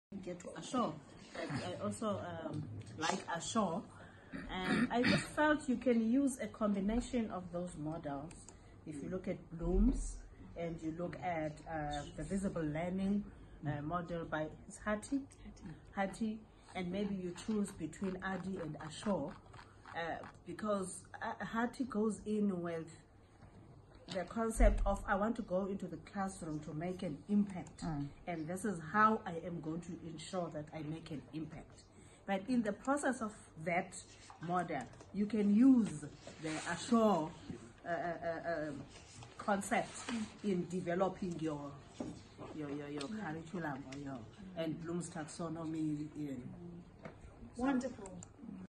2019.02.13.Discussion of Learning Models
As part of the Learning Design Workshop for CoL's Teacher Futures programme, Siyanda Ntlabathi provides her views on the various learning models - Hattie, Bloom, Assure and Addie.